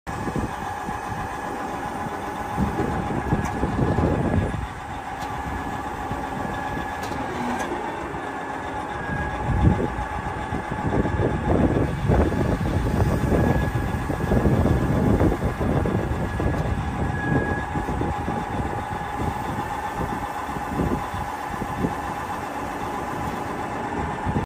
2020 DEERE 844L For Sale
2020 Deere 844L Wheel Loader S/N 705027, Only 2,386 Hours! Great Condition, Well maintained machine, 8.0 cu yard Bucket, Also available Sorting Grapple, 29.5 Michelin Tires